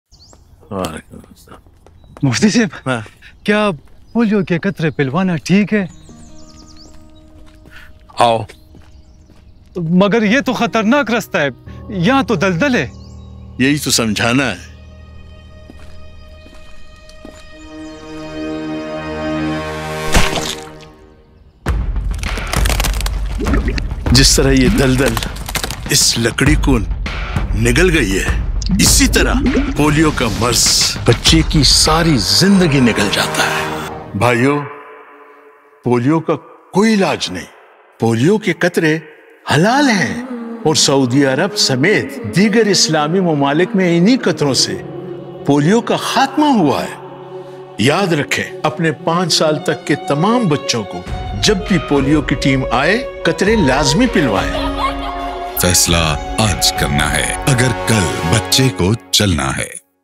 Polio Ad Mufti I Urdu | Polio Vaccination for our children’s health
In this powerful polio awareness ad, Mufti is explaining that Muslim countries, especially Saudi Arabia, have eradicated polio with the same vaccine along with assuring parents that polio drops are completely halaal. Mufti compares polio to a slow, destructive swamp that gradually takes away everything, symbolizing its impact on a child's life. Join us in the effort to eradicate polio and ensure a better future for our children.